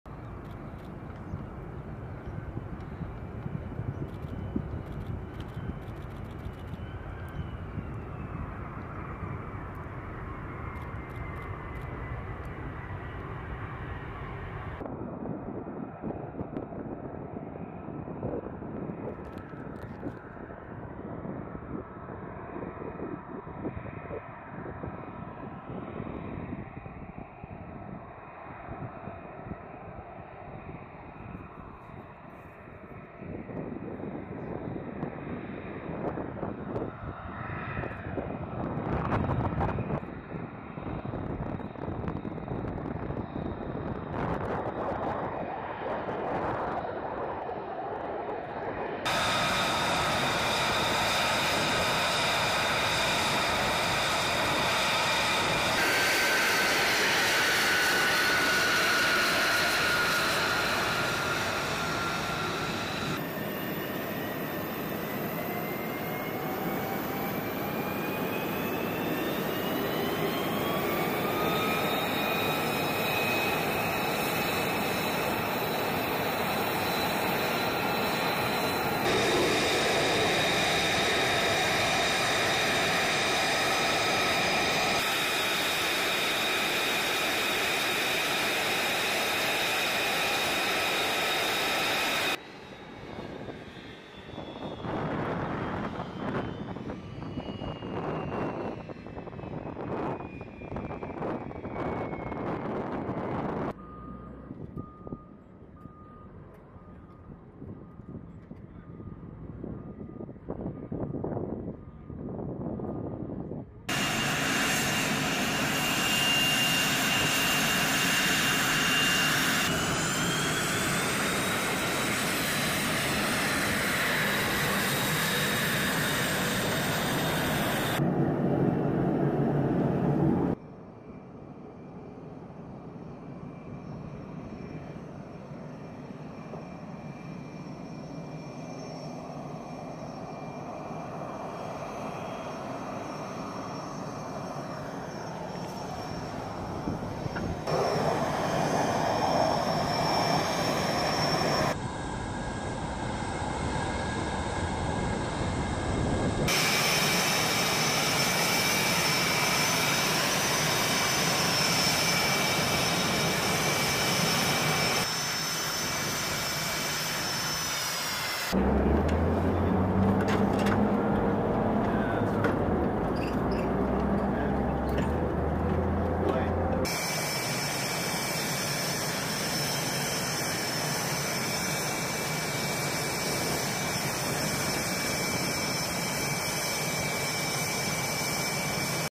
岩国基地のF-35B・F/A-18が韓国光州基地に到着 | VMFA-242/115 arrive at Gwangju Air Base for Korea Flying Training 23
韓国、光州航空基地
2023年4月12日
韓国の光州航空基地で行われる2023年度韓国飛行訓練（KFT23）に到着したい岩国基地の第242海兵隊戦闘攻撃飛行隊（VMFA-242）のF-35BライトニングIIとVMFA-115のF/A-18ホーネット。KFT23は、軍事的即応性を維持するための戦闘任務の戦術的遂行に焦点を当てた複合訓練イベントであり、米韓同盟の通常の年次訓練プログラムの一部です。

GWANJU AIR BASE, 29, SOUTH KOREA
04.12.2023
Video by Cpl. Ryan Ulibarri
1st Marine Aircraft Wing
U.S. Marine Corps F-35B Lightning II aircraft with Marine Fighter Attack Squadron (VMFA) 242 and F/A-18 Hornet aircraft with VMFA-115, arrive for the Fiscal Year 2023 Korea Flying Training at Gwangju AB, ROK, April 12, 2023. KFT 23 is a combined training event focused on tactical execution of combat missions to maintain military readiness and is part of the ROK-U.S. alliance’s routine, annual training program.